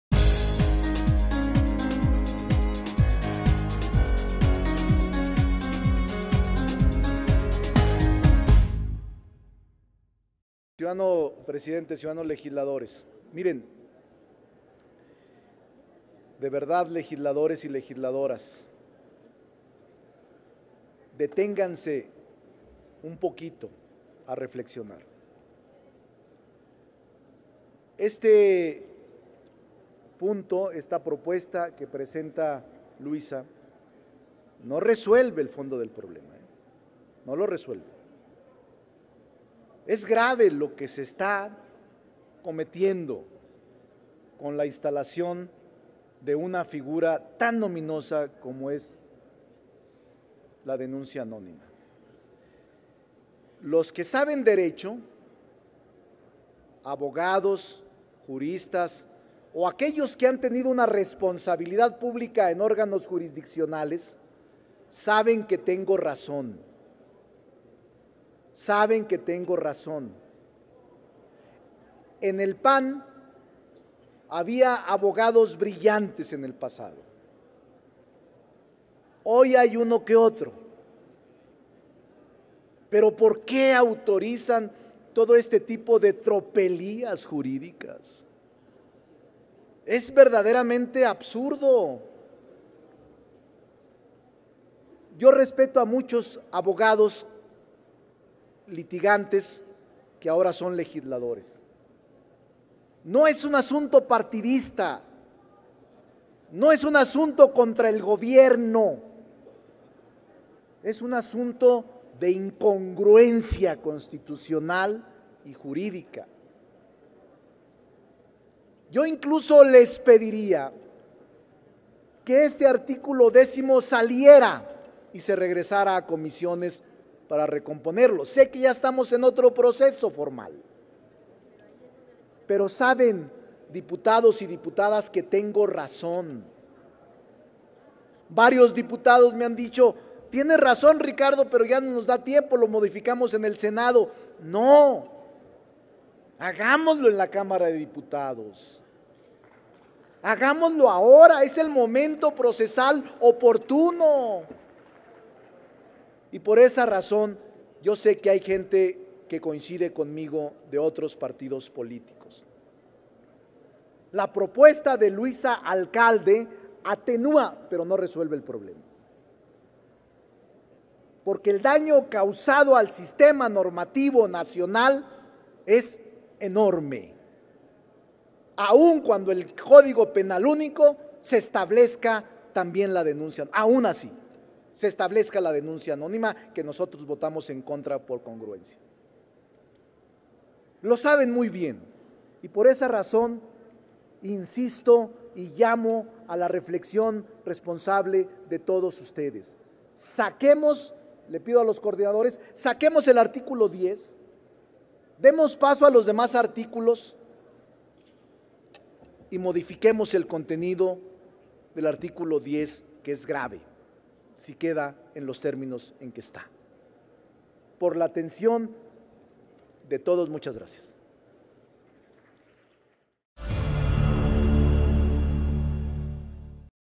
Dip. Ricardo Monreal (MC) - Ley Federal de Responsabilidades Administrativas (Razonar el voto)
El Dip. Ricardo Monreal Ávila (MC) señala que la propuesta presentada por la Dip. Luisa María Alcalde Luján (MC) atenúa pero no resuelve el tema de la denuncia anónima y reitera la -tropelía jurídica- que representa la reforma. Advierte que se trata de un asunto de incongruencia constitucional y solicita regresar el dictamen a la comisión dictaminadora. Finalmente, insiste en eliminar el artículo 10 del proyecto.